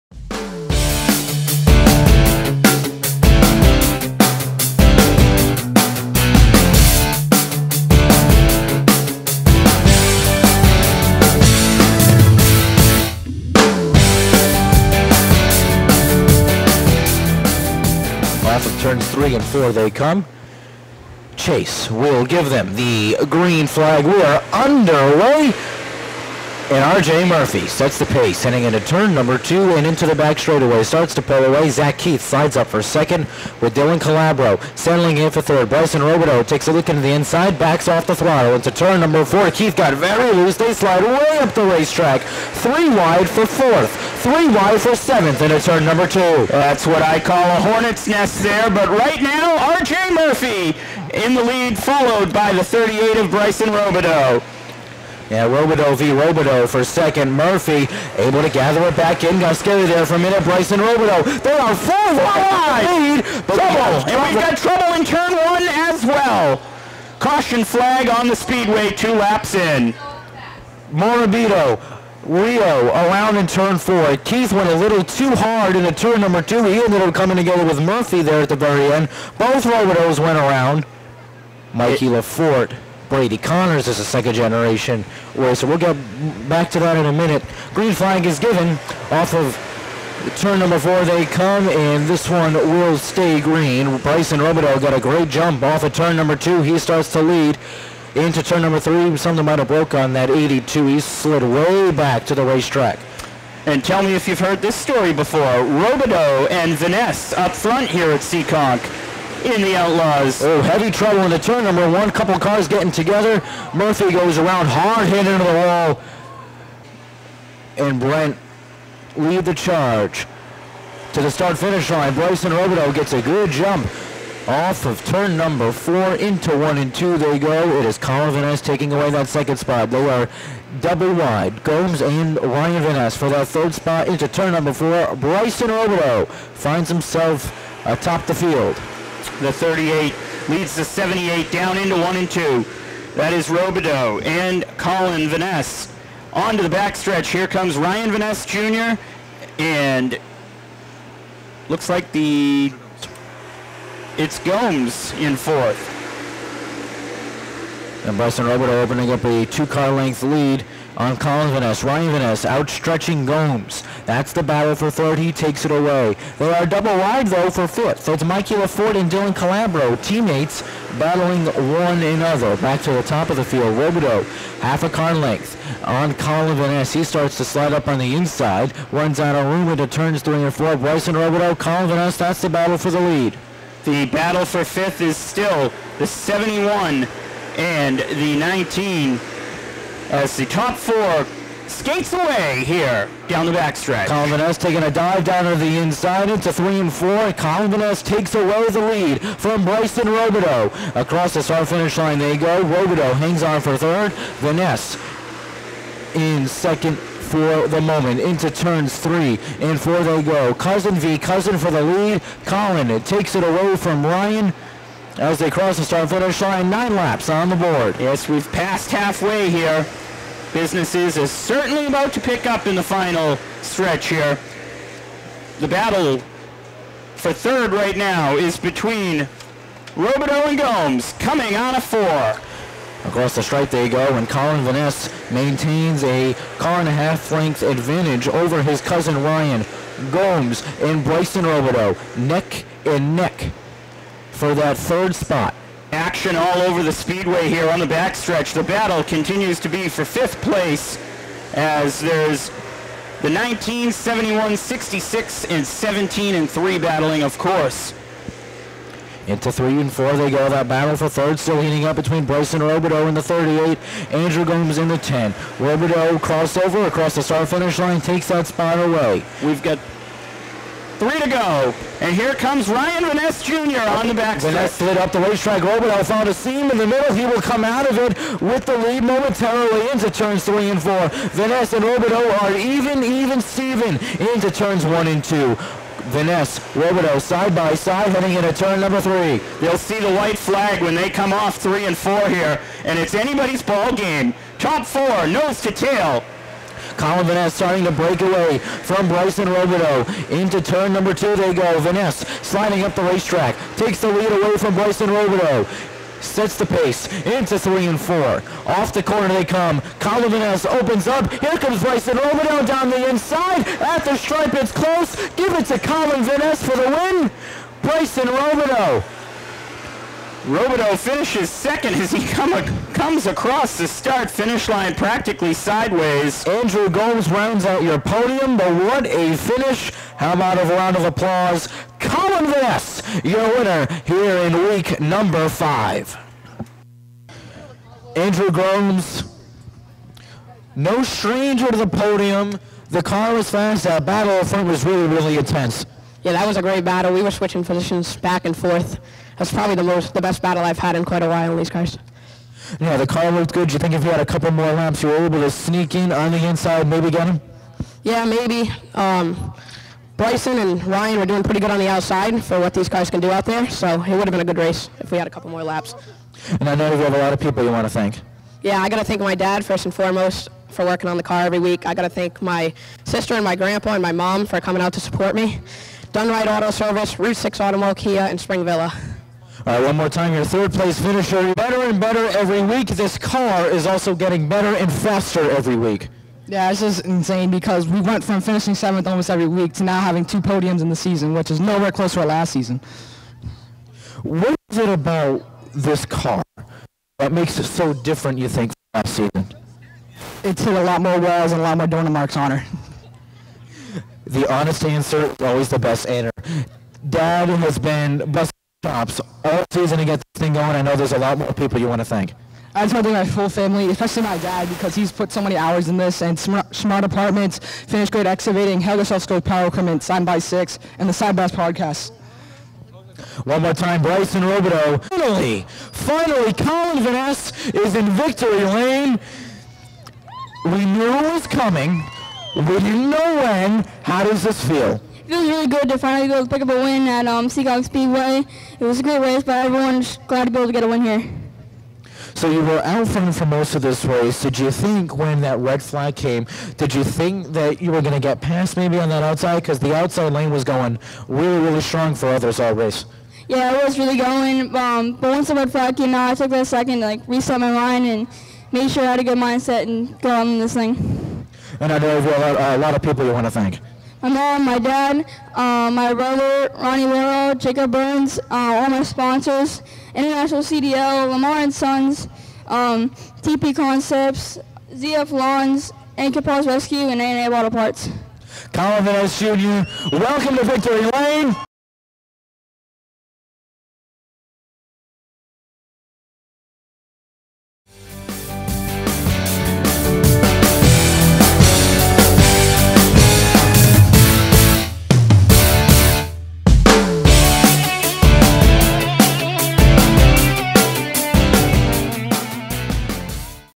7.7.23 Seekonk Speedway Outlaw Feature
Collin Vanasse finally was able to knock the monkey off his back Friday night in the Seekonk Grand Prix Bandolero Outlaws. In round two of the Everett’s Auto Parts Triple Crown Series, Vanasse held off all challengers during a four-car dash to the finish to score his first career victory.

Although a few of the early leaders tangled, many were able to continue because they tangled after a caution for another incident had already come out. Bryson Robidoux led laps three through seven, then Vanasse led laps nine through 12. On lap 13, Robidoux edged back out front. Then Vanasse took control and didn’t give it back.

Collin’s first career win came after many years of trying. Andrew Gomes finished third behind Vanasse and Robidoux. Ryan Vanasse Jr was fourth and Dylan Calabro fifth. Mikey LeFort III, Ben Morabito, Zachary Keith, Nathan Rioux and Zacarias Kelley finished the top-10.